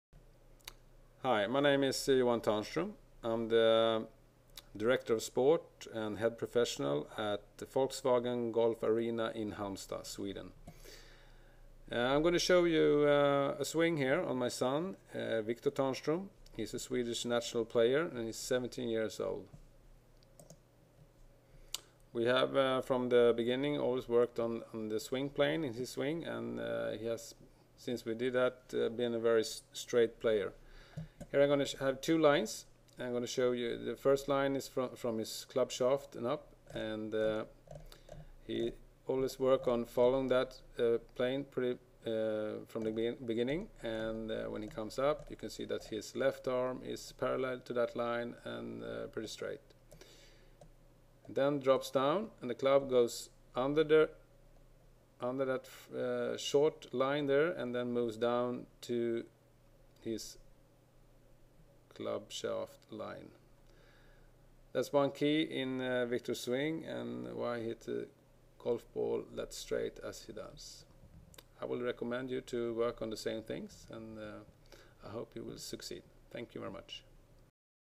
Swing of the week - Johan Tärnström with Victor Tärnström
This lesson was recorded at the magnificent VW Golfarena in Sweden